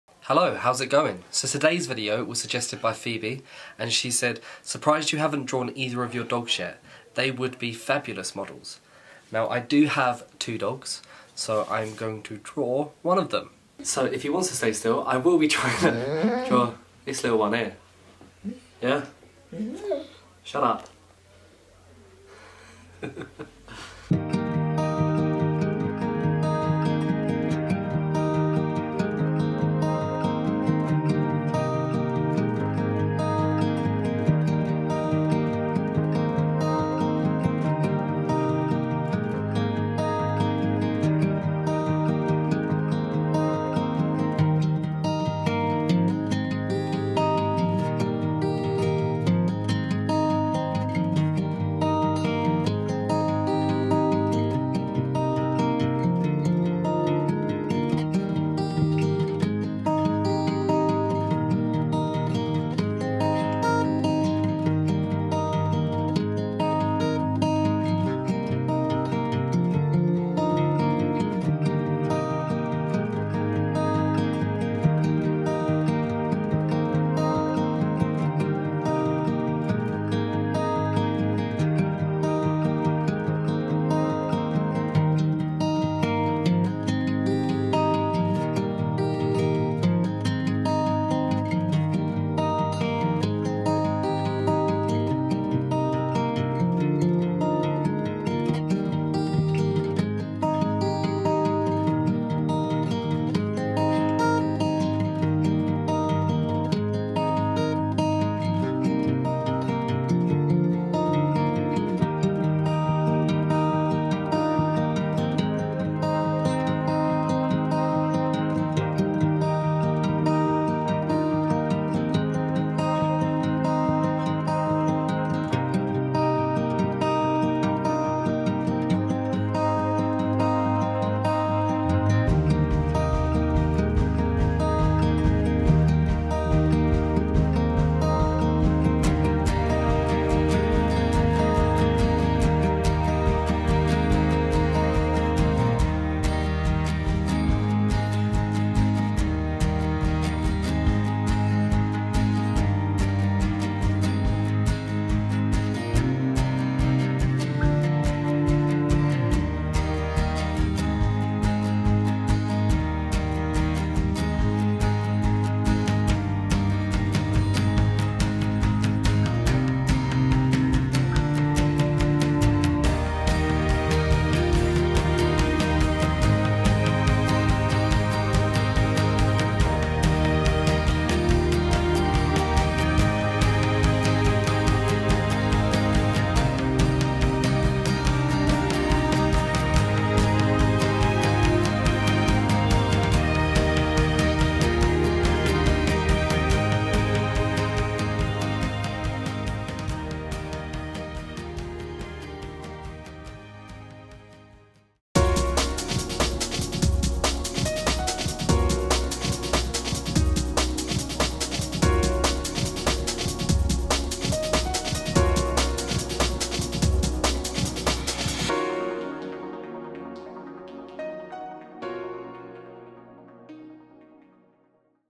Brodie
I draw one of my dogs

If you would like to make a donation for my charity swim, that would be unbelievable and you can do so